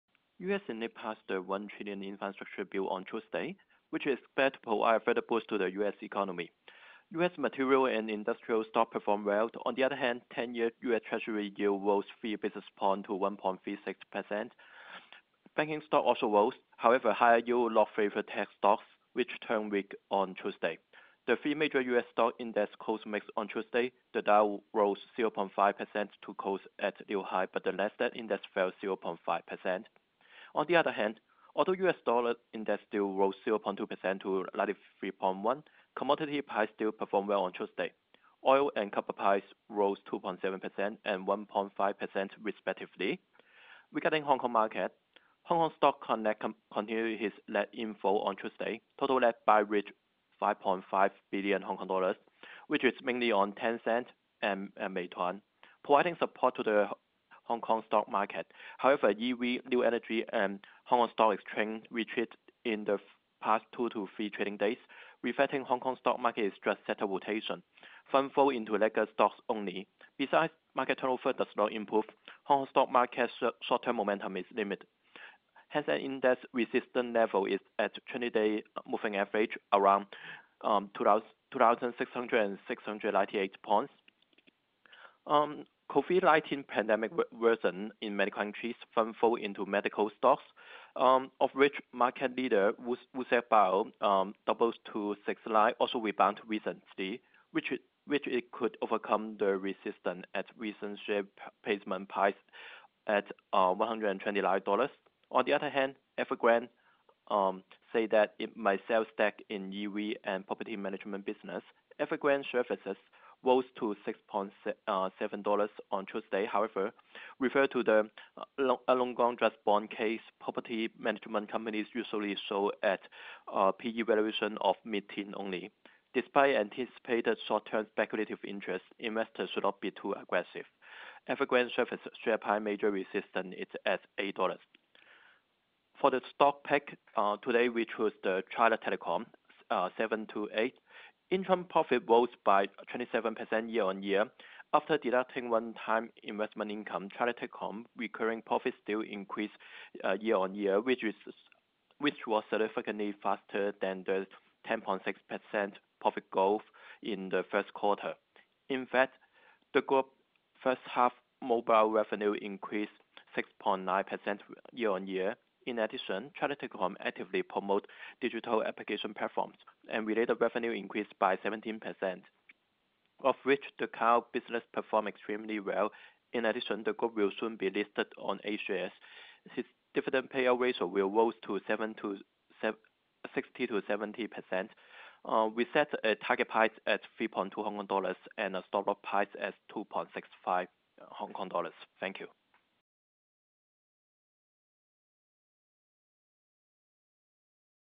Sector Rotation in Hong Kong stock market; Resistance at 20 Day SMA
KGI Morning Briefing-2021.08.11 ChinaTelecom (728)
Stock Pick
China Telecom (0728): Interim profit rose by 27.2% yoy. After deducting one-time investment income, China Telecom's recurring profit still increased yoy, which was significantly faster than the 10.6% profit growth 1Q21. In fact, the Group's 1H21 mobile revenue increased 6.9% yoy. In addition, China Telecom actively promoted digital application platforms, and related revenue increased by 16.8% yoy.  Of which, the cloud business performed extremely well. In addition, the group will soon be listed on A shares, it dividend payout ratio will rise to 60-70. Target price: HK$3.20, stop loss price: HK$2.65.